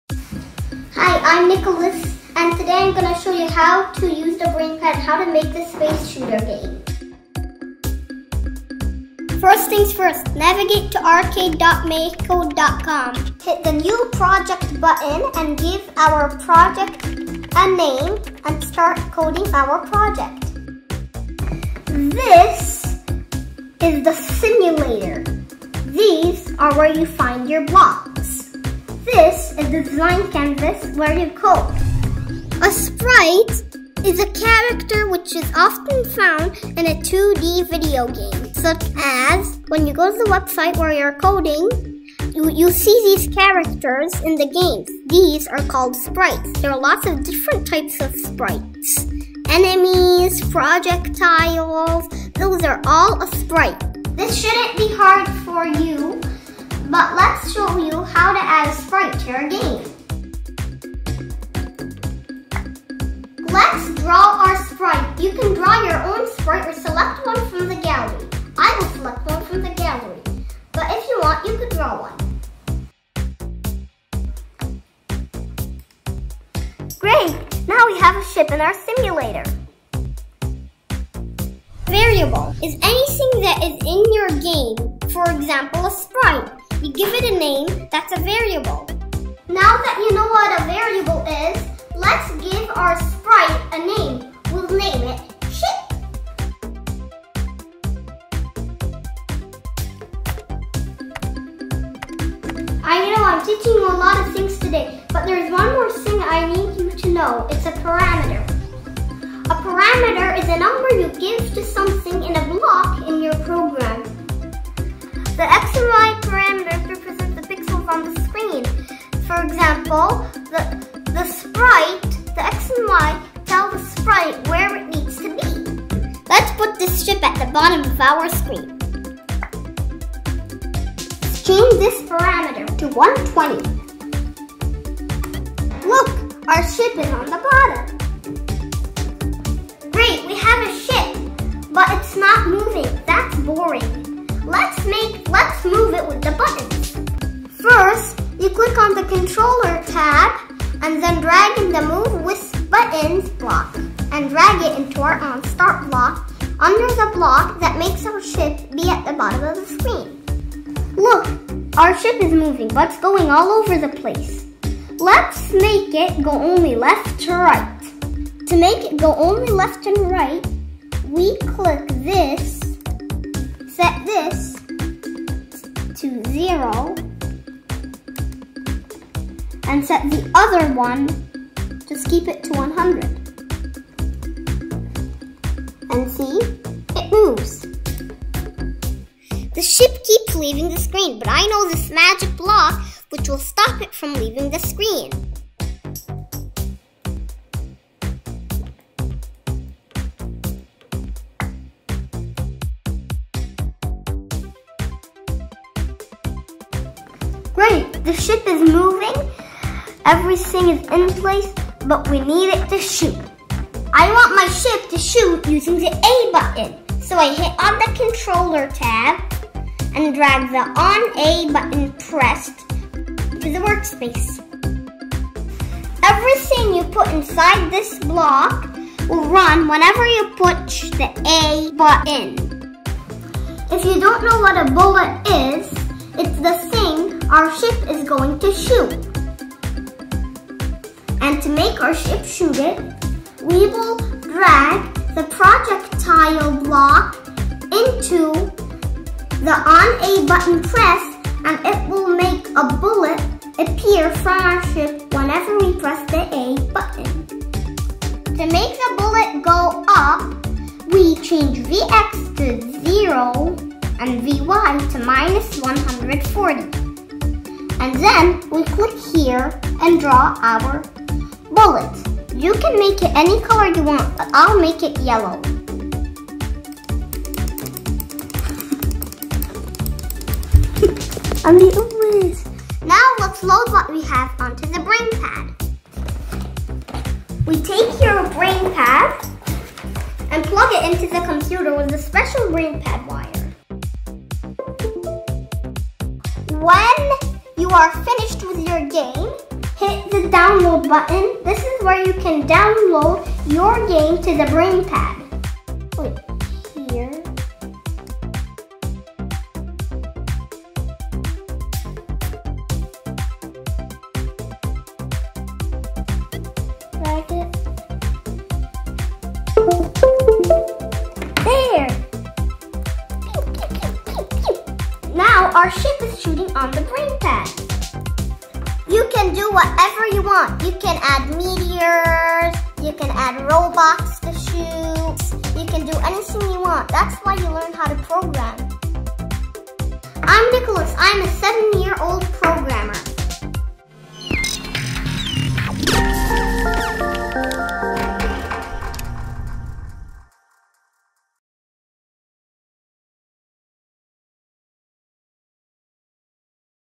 BrainPad Arcade: Space Shooter Game - By Nicolas
Nicolas shows us how to code a Space Shooter Game on the BrainPad using Microsoft MakeCode